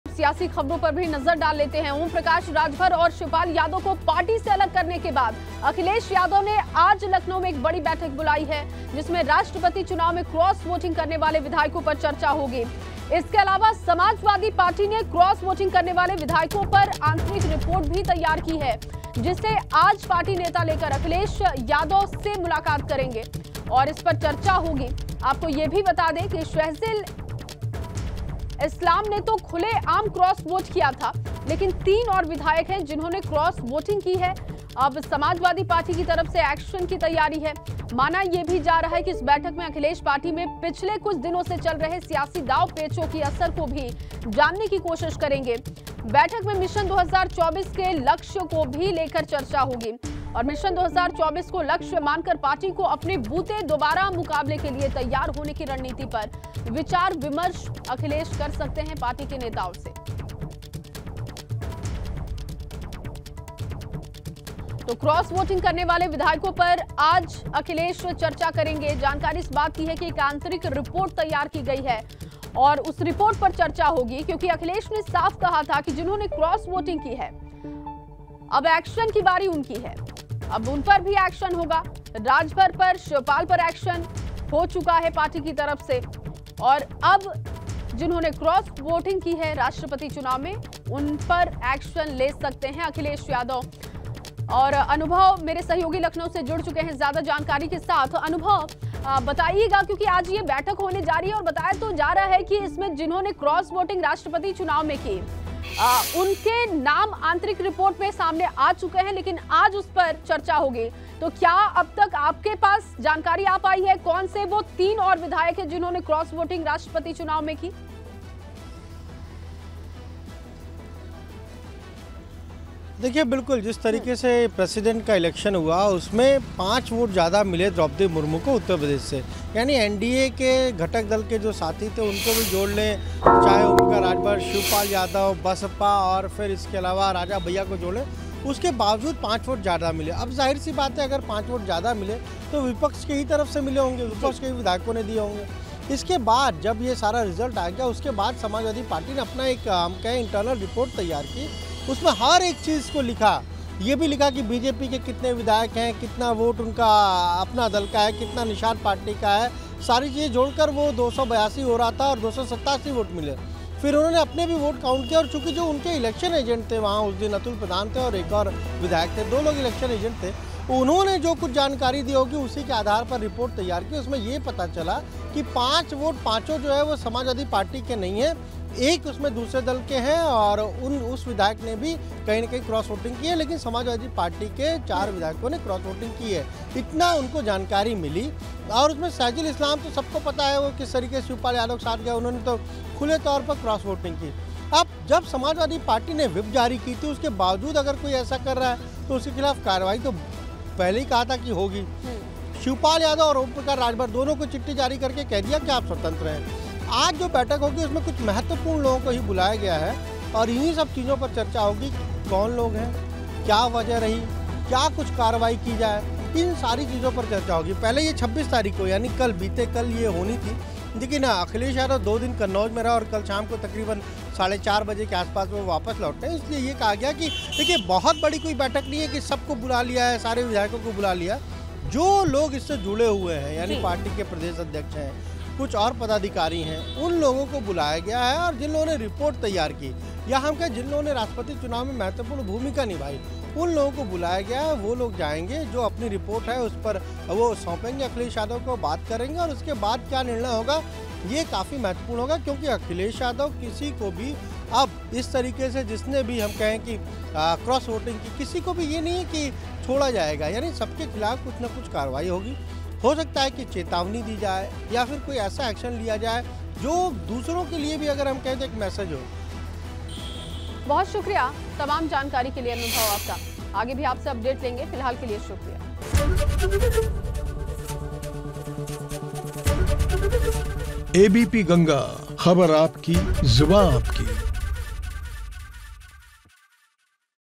Akhilesh Yadav की स्पेशल मीटिंग, क्रॉस वोटिंग के 'गुमनाम' की खोज! | OP Rajbhar
राष्ट्रपति चुनाव में क्रॉस वोटिंग करने वाले विधायकों पर चर्चा की जाएगी. इसके लिए समाजवादी पार्टी ने क्रॉस वोटिंग करने वाले विधायकों पर आंतरिक रिपोर्ट तैयार की है. जिसे आज पार्टी नेता लेकर अखिलेश यादव से मुलाकात करेंगे और इसपर चर्चा करेंगे..